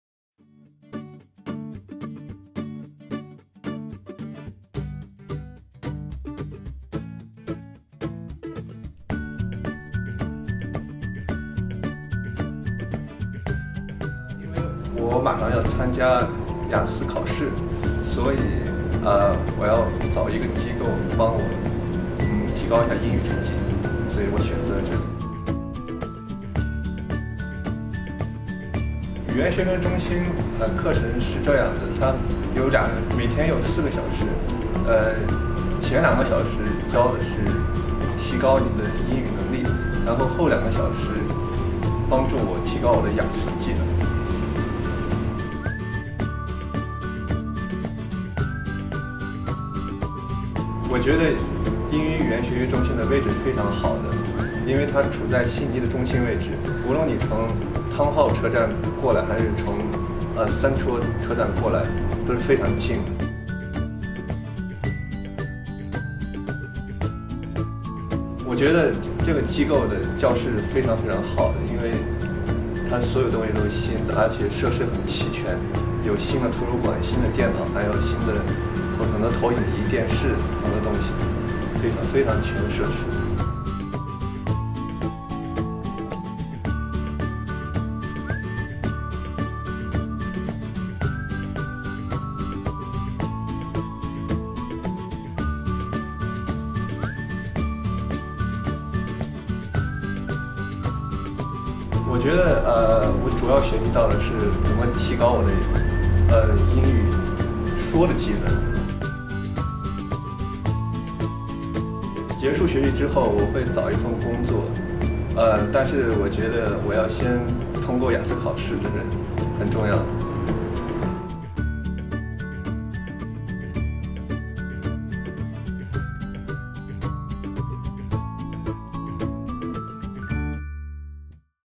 南天大學『英語語言中心』的學習之旅-學生采访：Vince
What is like to study English at the NTI English Language Centre? We had a chat to one of our student's - Vincent - and asked him about his experience. We were very proud to learn that he achieved the IELTS result he was after and as a result was able to gain work in Sydney - well done!